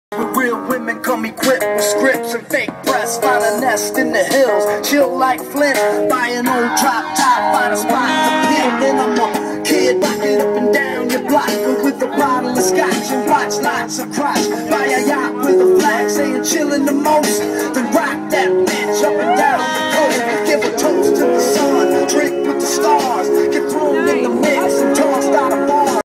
Blinging Harley
Devas bling moving her bike at Arizona bike week 2011 in scottsdale.